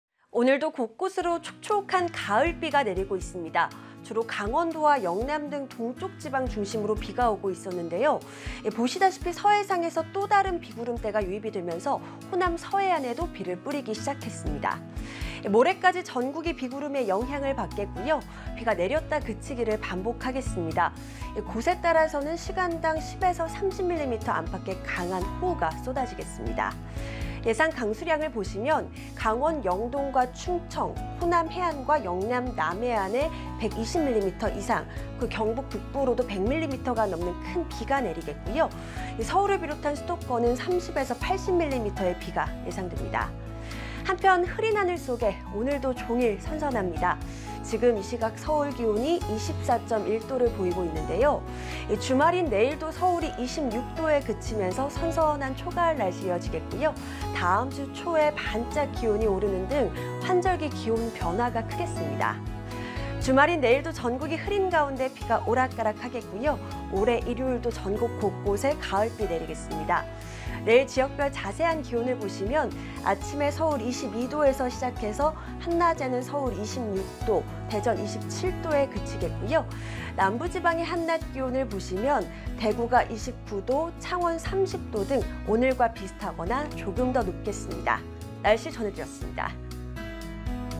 [날씨] 주말도 전국 오락가락 비…종일 선선 / 연합뉴스TV (YonhapnewsTV)
[날씨] 주말도 전국 오락가락 비…종일 선선

오늘도 곳곳으로 촉촉한 가을비가 내리고 있습니다.

주로 강원도와 영남 등 동쪽지방에 비가 오고 있었는데요.

보시다시피 서해상에서 또다른 비구름대가 유입되면서, 호남서해안에도 비를 뿌리기 시작했습니다.

모레까지 전국이 비구름의 영향을 받겠고요.

비가 내렸다, 그치기를 반복하겠습니다.

곳에 따라서는 시간당 10-30mm 안팎의 강한 호우가 쏟아질텐데요.

예강강수량을 보시면, 강원영동과 충청, 호남해안과 영남남해안에 많게는 120mm 이상, 경북북부로도 100mm가 넘는 큰 비가 내리겠고요.

서울을 비롯한 수도권은 30에서 80mm의 비가 예상됩니다.

한편, 흐린 하늘 속에, 오늘도 종일 선선합니다.

지금 이 시각 서울 24.1도 보이고 있는데요.

주말인 내일도 서울 26도에 그치면서, 선선한 초가을 날씨가 이어지겠고요.

다음주 초에 반짝 기온이 오르는 등 환절기 기온 변화가 크겠습니다.

주말인 내일도 전국이 흐린 가운데 비가 오락가락하겠고요.

모레 일요일도 전국 곳곳에 가을비가 내리겠습니다.

내일 지역별 자세한 기온을 보시면 아침에 서울 22도에서 시작해서 한낮에는 서울 26도, 대전 27도에 그치겠고요.

남부지방의 한낮 기온을 보시면 대구가 29도, 창원 30도 등 오늘과 비슷하거나 조금 더 높겠습니다.

(구본아 기상캐스터)